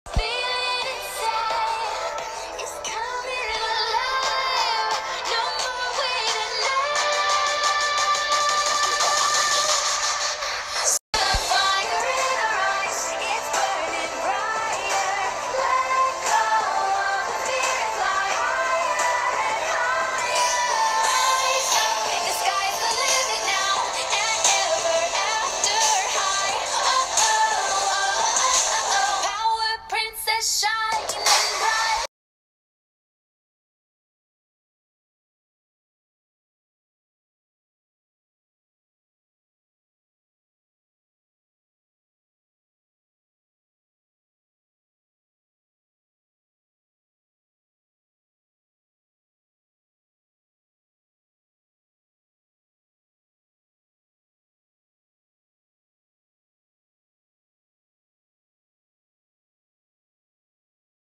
You guys Learn something about me￼ || this is real UwU ￼
I respect my friend for being gay you can hate me all you Want￼